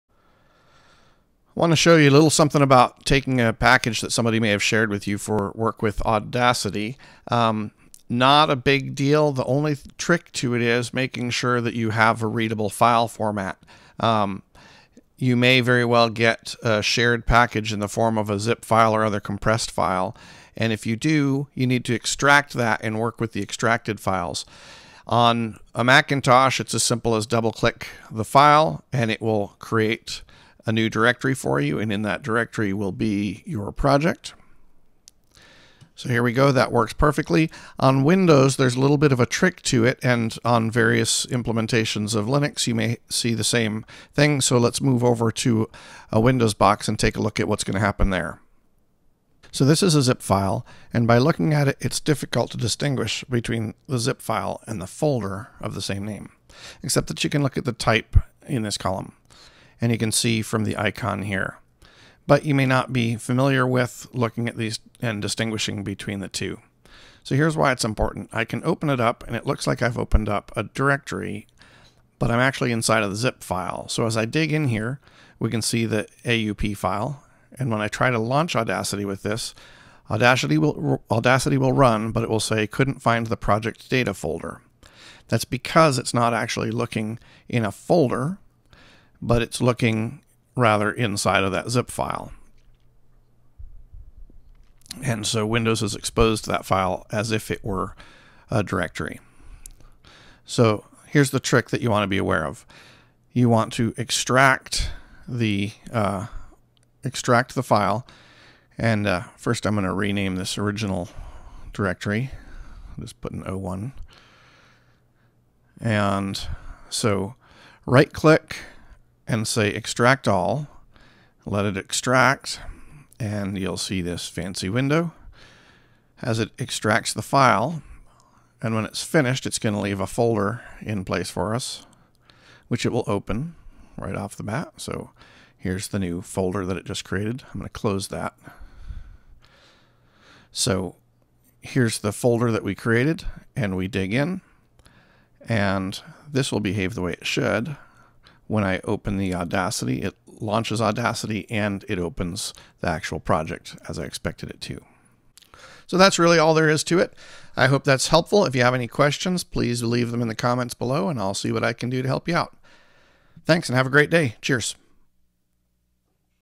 Opening Shared Audacity File
If someone shares an Audacity package with you, it is likely shared as a compressed file.  Make sure to extract first, and use the extracted version of the Audacity package.